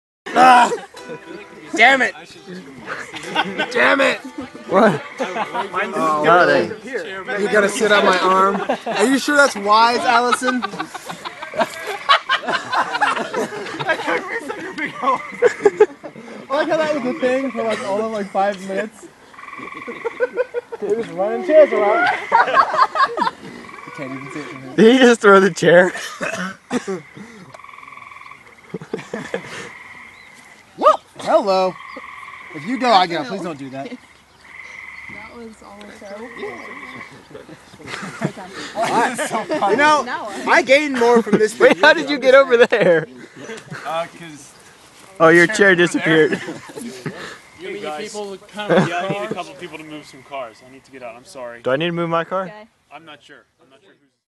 Just Goofing Around With Some Folding Chairs (Don't Mind Us)
Graduation day of highschool...wonderful.
Someone started knocking folding chairs over when someone got up, and it just escaladed into running through the field strewing chairs about.